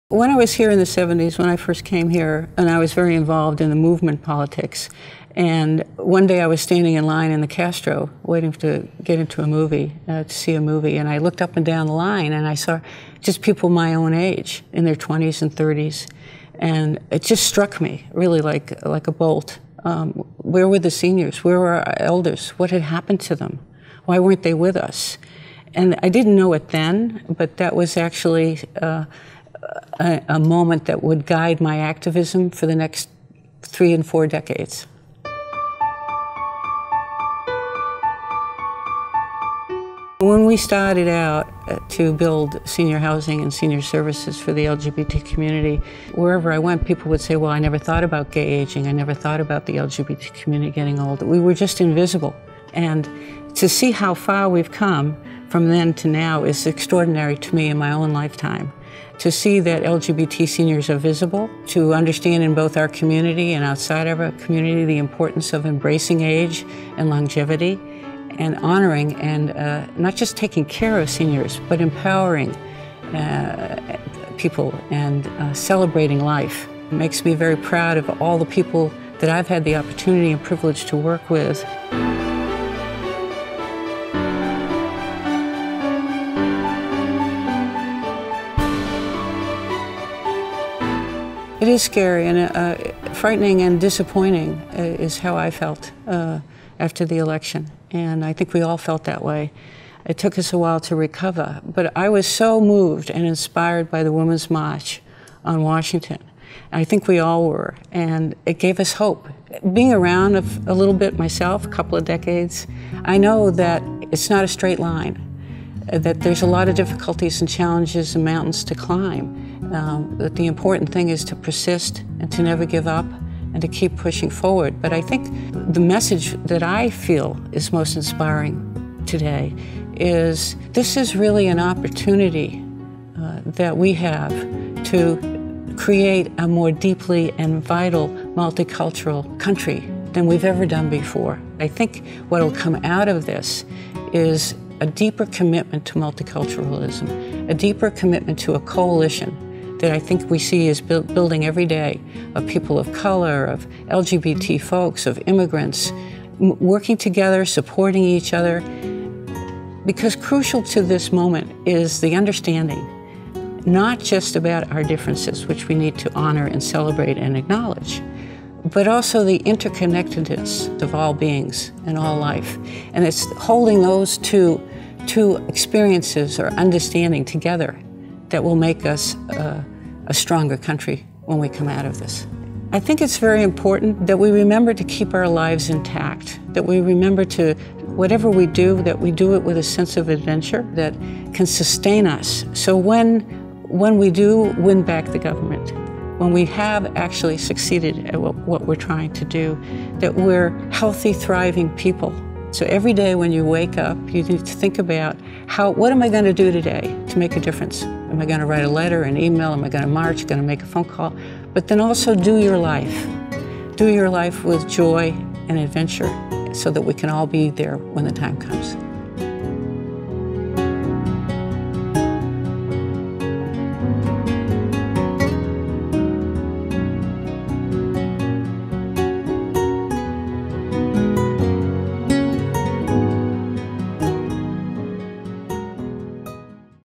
SF Pride 2017 Lifetime Achievement Award: Marcy Adelman
Dr. Marcy Adelman is a widely respected psychotherapist and pioneer in the field of lesbian and gay aging. In 1998, many couldn’t afford rising rents or were evicted and senior housing and service providers didn’t know how to welcome LGBT elders and assure their safety. With few housing options, LGBT elders had to go back into the closet to access housing and services or were forced to leave the city. Marcy and her late partner Jeannette Gurevitch founded Openhouse to address the housing and service needs of LGBT elders. For over three decades, Marcy has worked to make visible the lives, contributions, and concerns of LGBT elders and is a leading advocate for quality LGBT affirming elder care and policies.